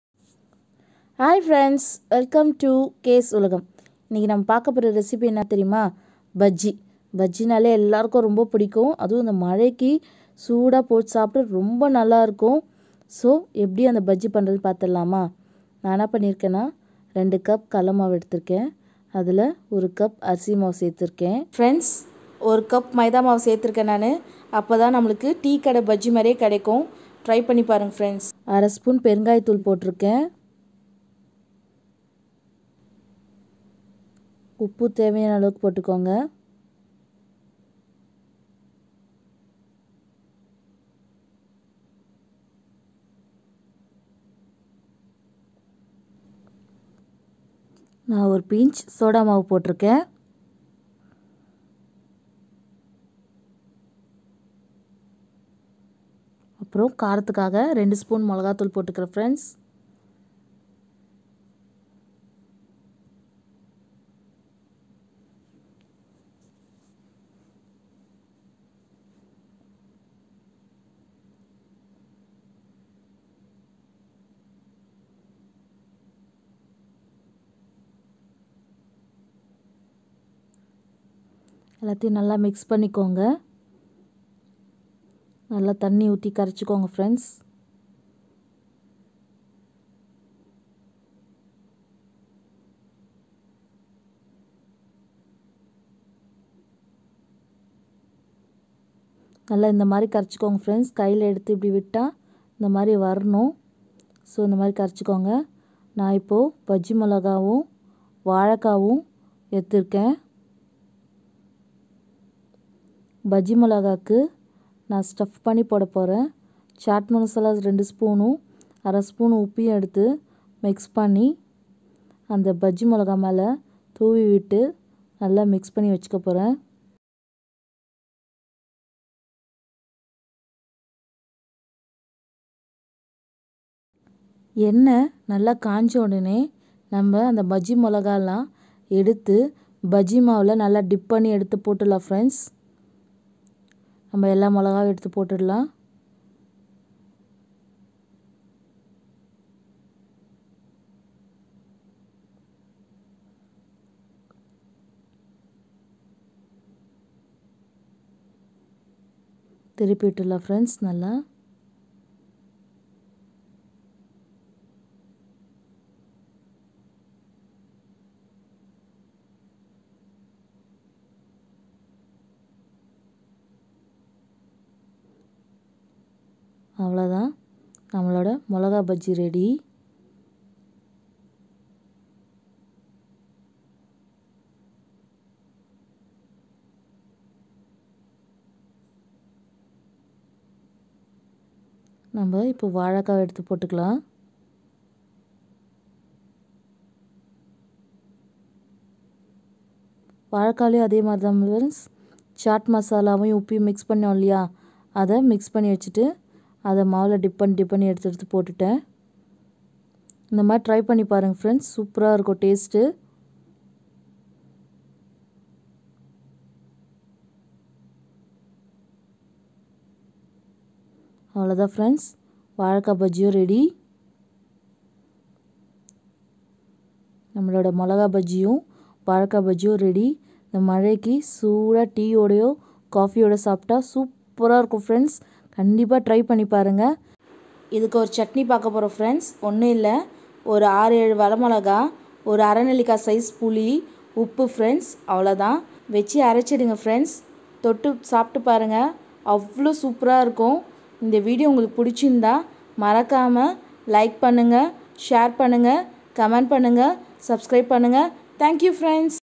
How to make a Tea kadai Bajji Recipe/valaikai bajji and chilly bajji recipe in tamil
tea kadai Bajji recipe in tamil
chilly bajji recipe & valaikai bajji recipe

ingredients
maida 1cup
gram dal flour 2 cup
Salt taste
chilli 4 to 6 cut into 2 pieces

chilli bajji
valaikai bajji
bajji recipe